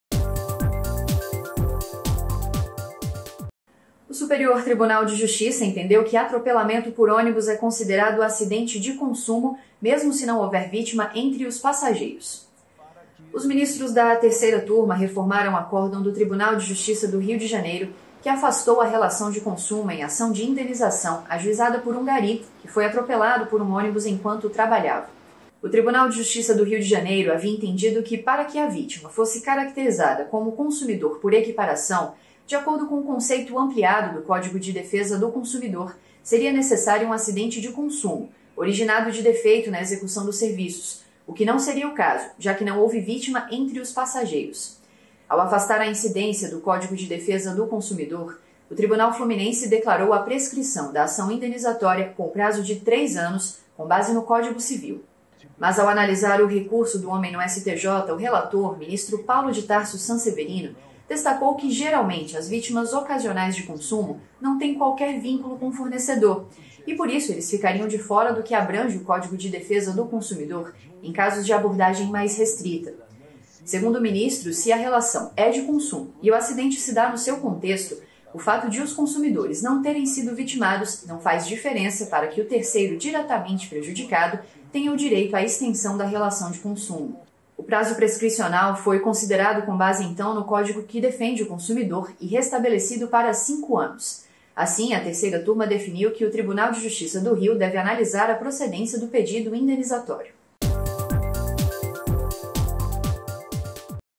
Atropelamento por ônibus é acidente de consumo mesmo não havendo vítimas entre os passageiros 31.07
​​​​Com a aplicação do conceito ampliado de consumidor estabelecido no artigo 17 do Código de Defesa do Consumidor – conhecido como bystander –, a Terceira Turma do Superior Tribunal de Justiça (STJ) reformou acórdão do Tribunal de Justiça do Rio de Janeiro (TJRJ) que afastou a relação de consumo em ação de indenização ajuizada por um gari atropelado por ônibus enquanto trabalhava.
REsp 1787318